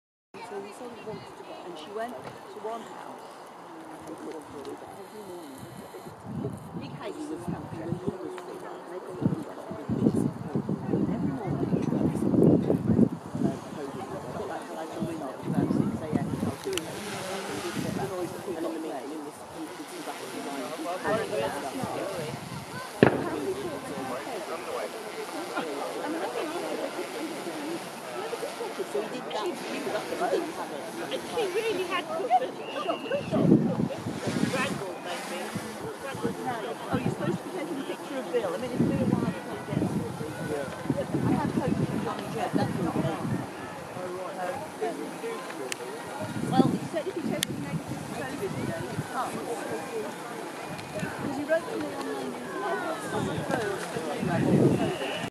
18 18 P1060179
2022 Cambridge Bumps.
Filmed from Osier Holt by Polly McGinty 22nd July 2022